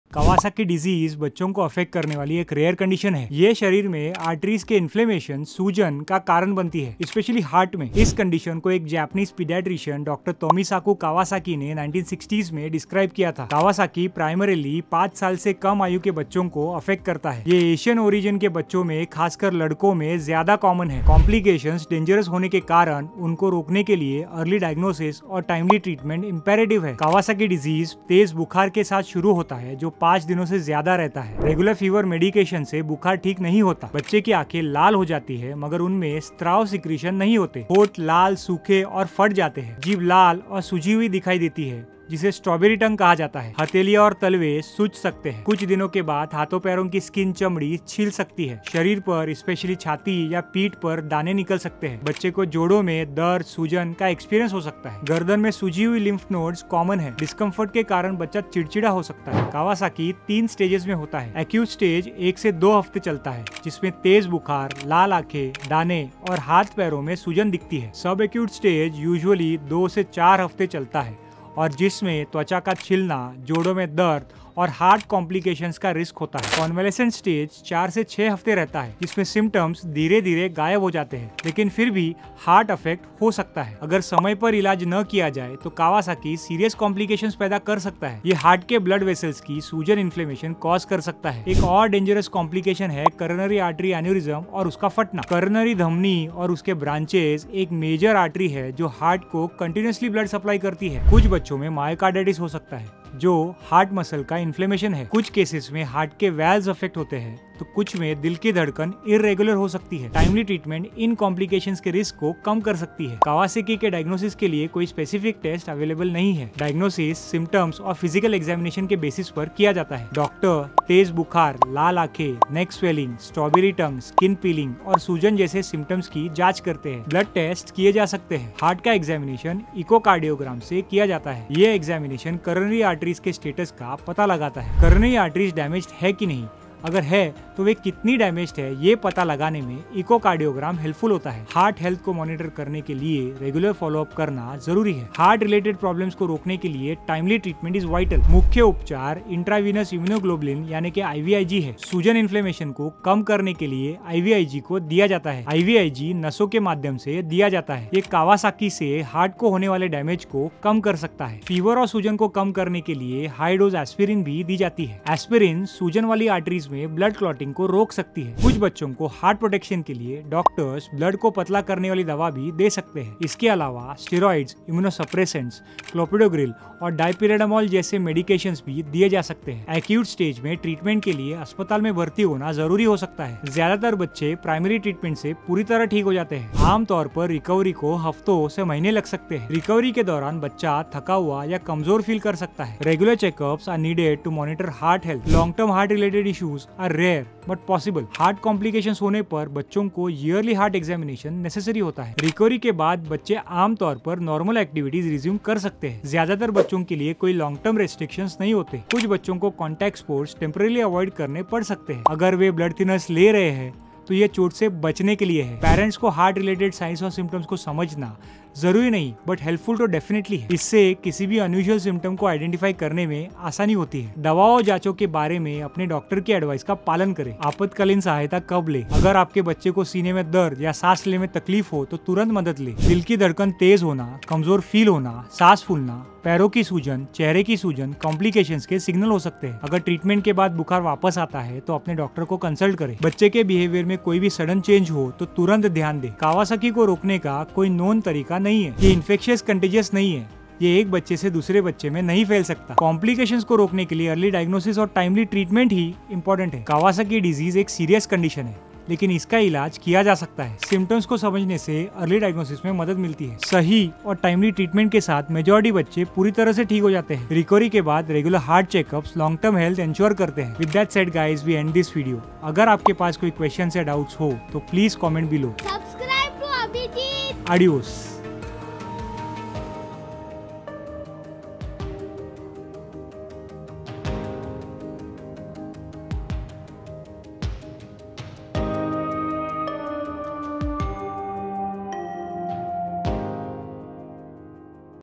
Kawasaki Disease, (causes symptoms treatment) explained in Hindi
- **Kawasaki disease** is a rare condition affecting children.
- It causes inflammation in the blood vessels throughout the body.
- This inflammation mainly affects the heart's arteries.
- The exact cause of Kawasaki disease is still unknown.

Who is Affected by Kawasaki Disease?
- Kawasaki disease primarily affects children under 5 years.
- It is more common in boys than in girls.
- It is also more common in children of Asian descent.
- It can happen in any race or ethnicity.
- Early diagnosis and treatment are essential to prevent complications.

Symptoms of Kawasaki Disease
- Kawasaki disease starts with a high fever lasting more than 5 days.
- The fever does not improve with regular medicine.
- The child may have red eyes without discharge.
- The lips become red, dry, and cracked.
- The tongue may appear red and swollen, called "strawberry tongue."
- The palms of the hands and soles of the feet may swell.
- The skin on the hands and feet may peel after some days.
- A rash may appear on the body, usually on the chest or back.
- The child may experience joint pain or swelling.
- Swollen lymph nodes, especially in the neck, are common.
- The child may be irritable and fussy due to discomfort.

Stages of Kawasaki Disease
- Kawasaki disease occurs in three stages.

  1. **Acute Stage** (1-2 weeks):
     - High fever, red eyes, rash, and swollen hands/feet.
  2. **Subacute Stage** (2-4 weeks):
     - Peeling skin, joint pain, and risk of heart issues.
  3. **Convalescent Stage** (4-6 weeks):
     - Symptoms slowly disappear but may still affect the heart.

Complications of Kawasaki Disease
- If untreated, Kawasaki disease can cause serious complications.
- It can lead to heart problems, including inflammation of heart vessels.
- **Coronary artery aneurysms** can form, which is life-threatening.
- Some children may develop **myocarditis**, an inflammation of the heart muscle.
- Heart valve problems or irregular heartbeats can occur in some cases.
- Early treatment can reduce the risk of these complications.

Diagnosing Kawasaki Disease
- There is no specific test to diagnose Kawasaki disease.
- Diagnosis is based on symptoms and a physical exam.
- Doctors will check for signs like a high fever and red eyes.
- Blood tests may be done to check for inflammation.
- An echocardiogram is often used to check the heart.
- This test helps detect any damage to the coronary arteries.
- Regular follow-ups are needed to monitor heart health.

Treatment for Kawasaki Disease
- Early treatment is crucial for preventing heart problems.
- The main treatment is **intravenous immunoglobulin** (IVIG).
- IVIG is given through a vein to reduce inflammation.
- It helps lower the risk of heart damage from the disease.
- High-dose **aspirin** is also given to reduce fever and inflammation.
- Aspirin helps prevent blood clots in the inflamed arteries.
- Some children may need blood-thinning medication for heart protection.
- Hospitalization is often needed during the acute phase for treatment.

Recovery from Kawasaki Disease
- Most children recover fully with early treatment.
- Recovery usually takes several weeks to months.
- The child may feel tired or weak during recovery.
- Regular check-ups are needed to monitor heart health.
- Long-term heart problems are rare but possible.
- Children may need yearly heart tests if complications occurred.

Living with Kawasaki Disease
- After recovery, children usually return to normal activities.
- There are no long-term restrictions for most children.
- Some children may need to avoid contact sports temporarily.
- This is to prevent injury if they are taking blood thinners.
- Parents should watch for any signs of heart problems.
- Follow your doctor’s advice on medications and check-ups.

When to Seek Emergency Help
- Seek help if your child has chest pain or shortness of breath.
- Rapid heartbeats or extreme fatigue may be signs of complications.
- If the fever returns after treatment, contact your doctor.
- Any sudden changes in the child's behavior need immediate attention.

Prevention of Kawasaki Disease
- There is no known way to prevent Kawasaki disease.
- It is not contagious and cannot be passed from child to child.
- Early diagnosis and treatment are key to preventing complications.
- Always seek medical help if a fever lasts more than 5 days.

Conclusion
- **Kawasaki disease** is a serious condition, but it can be treated.
- Understanding the symptoms helps in early diagnosis.
- With the right treatment, most children recover fully.
- Regular heart check-ups ensure long-term health after recovery.